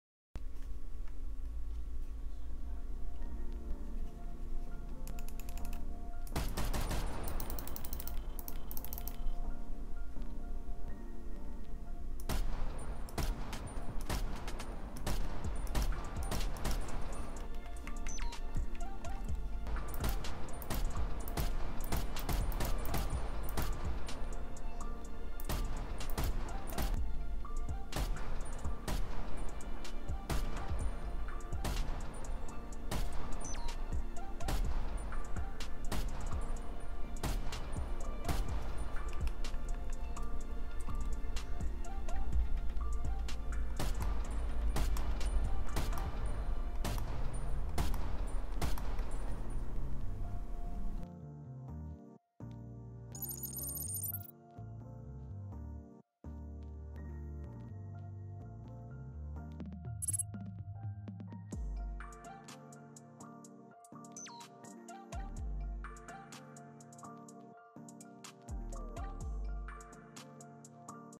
PS5 Dualsense Stock vs Mouse Clicks
Video to show the difference between a stock controllers bumpers/triggers vs. the mouse clicks or also known as "smart switches".